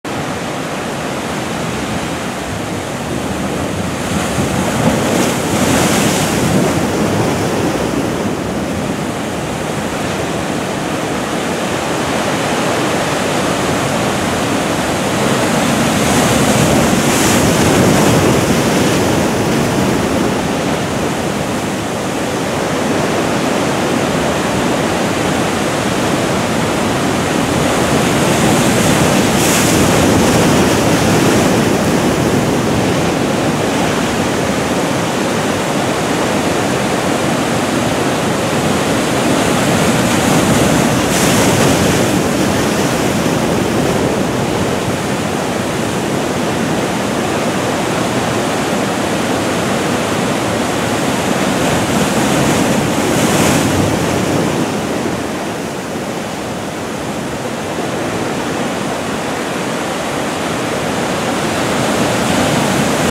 And this was at low tide!
Everywhere a rock, everywhere a wave, but all so different and breathtakingly beautiful. Oahu, near Ka'ena point.